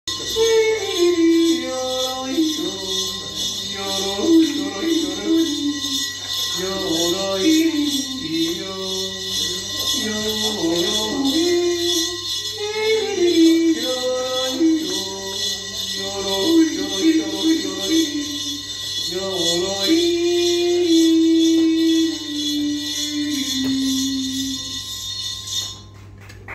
🗣️ Alpine yodeling
🗣️ Yodeling (also jodeling) is a form of singing which involves repeated and rapid changes of pitch between the low-pitch chest register (or "chest voice") and the high-pitch head register or falsetto. The English word yodel is derived from the German (and originally Austro-Bavarian) word jodeln, meaning "to utter the syllable jo" (pronounced "yo" in English). Although this vocal technique is used in many cultures worldwide, yodeling is usually associated with Alpine regions, such as Austria, Bavaria and Switzerland.

Shot on June 16, 2022, in Geneva, Switzerland 🇨🇭.